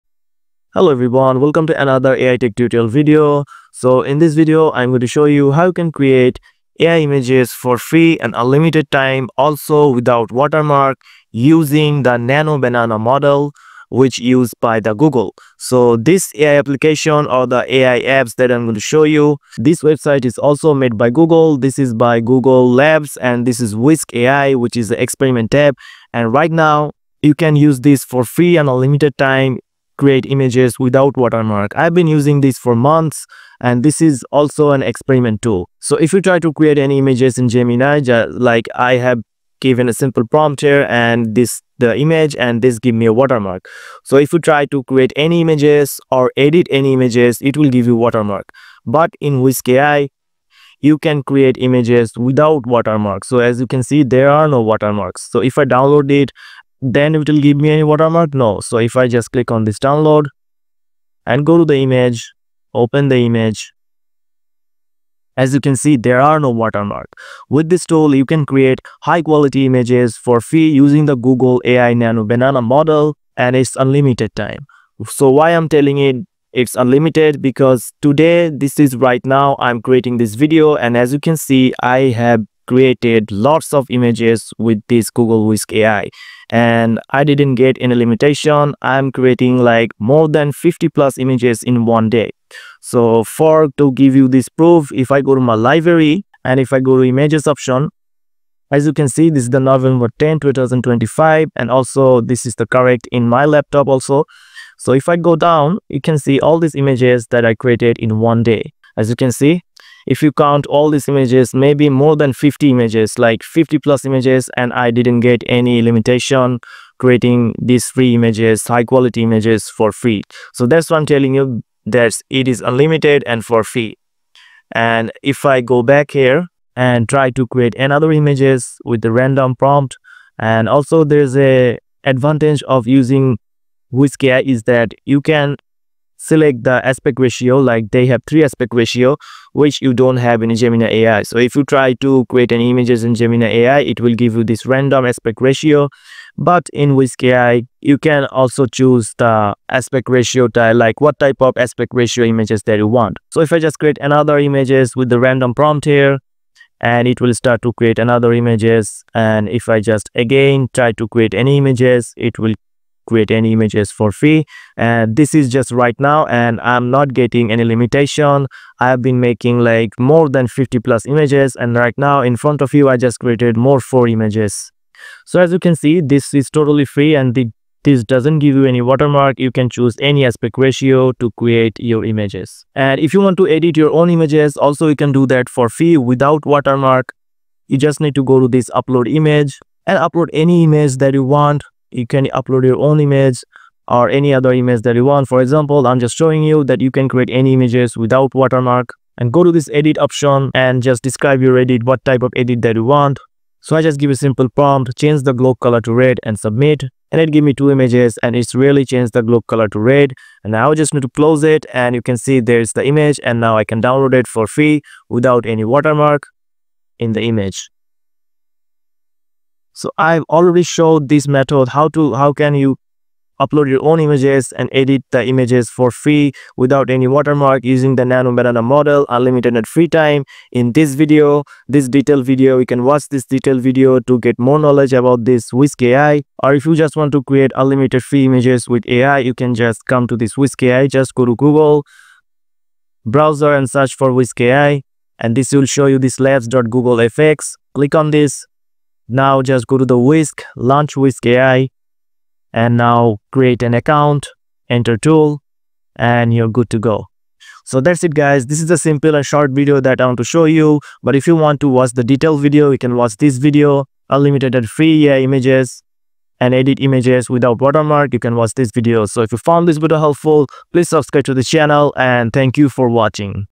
GOOGLE SECRET AI TOOL to Generate FREE & UNLIMITED AI Images NO WATERMARK | No Daily Limits
GOOGLE SECRET AI TOOL to Generate FREE & UNLIMITED AI Images NO WATERMARK | No Daily Limits - google whisk ai.
create gemini images without watermark and free.
You won't believe the images you can create with this FREE and UNLIMITED AI image generator also no watermark ! In this video, I'll show you how to use google whisk ai , an underrated AI tool that's giving Midjourney and SeaDream a run for their money.

In this tutorial, you'll learn:

How to get use google whisk ai  (it's easy!)

How to generate stunningly realistic, cinematic, and anime-style images from simple text prompts.

How to use the powerful image editing features with nanobanana  edit any image for free.

How to upload your own images and edit them with incredible consistency.

If you're tired of paying for AI image generation or hitting frustrating limits, you need to check this out.

more detail video on whisk ai -